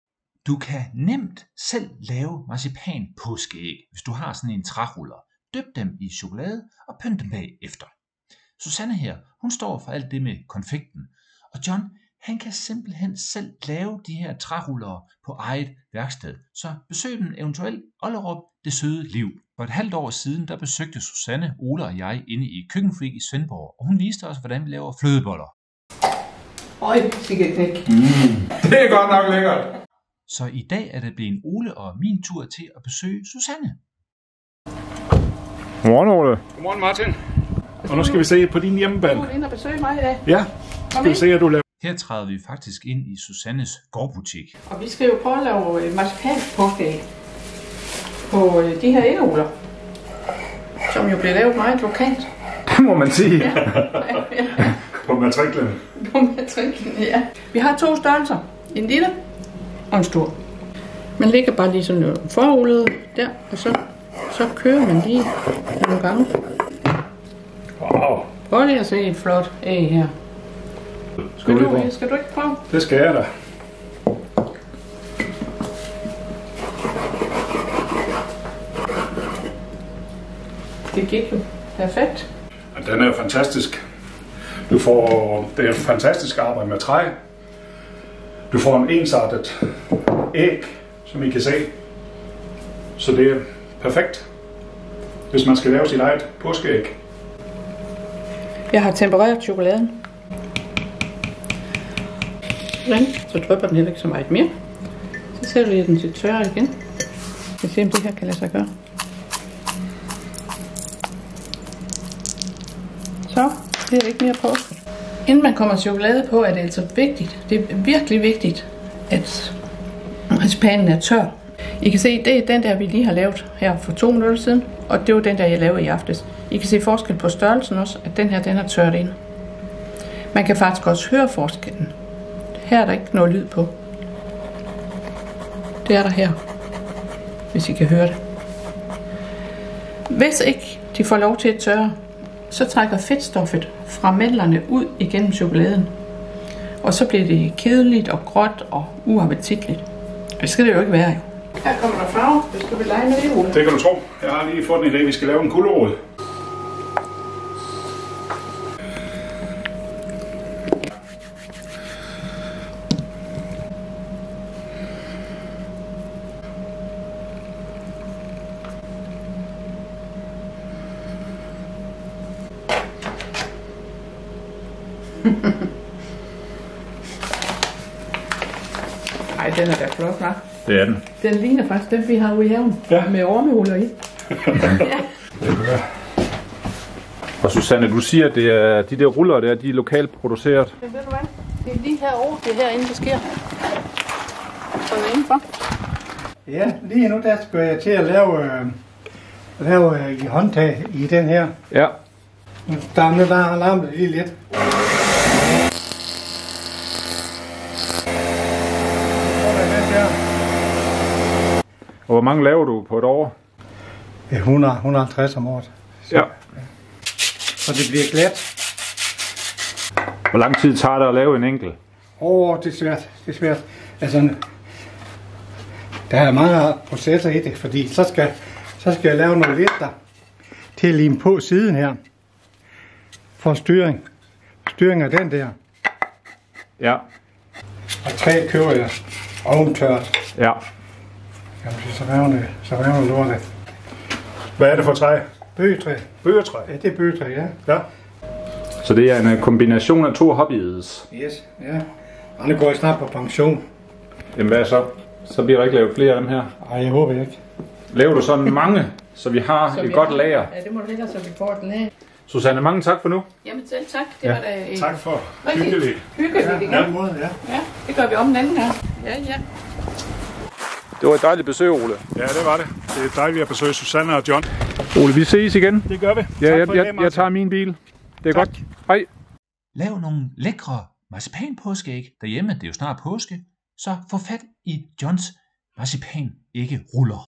Lav dine egne påskeæg af marcipan med denne træ-ruller
Det er nemt, sjovt og kanon lækkert. Du kan blande forskellige smage i. Det er hyggeligt at lave sammen med venner, familien, børnene.